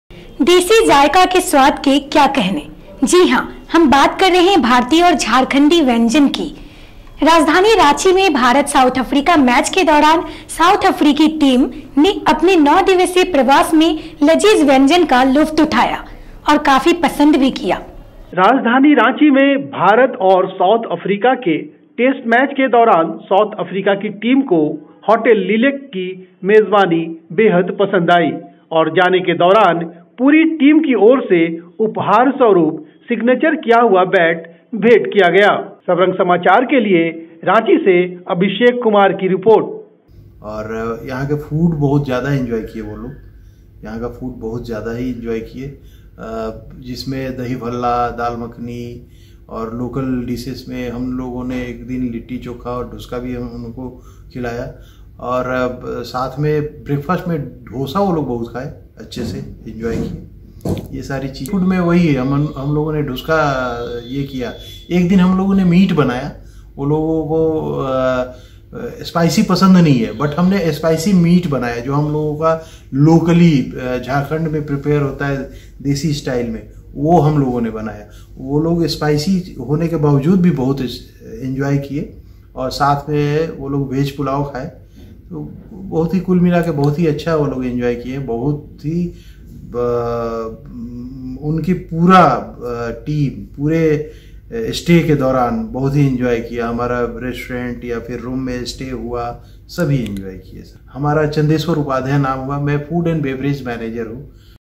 साउथ अफ्रीकी टीम को भाया देशी जायका
देशी जायका के स्वाद के क्या कहने ... जी हां हम बात कर रहे है भारतीय और झारखंडी व्यंजनों की राजधानी रांची में भारत साउथ अफ्रीका टेस्ट मैच के दौरान साउथ अफ्रीका की टीम ने अपने नौ दिवसीय प्रवास में लजीज व्यंजनों का लुफ्त उठाया और काफी पंसद भी किया .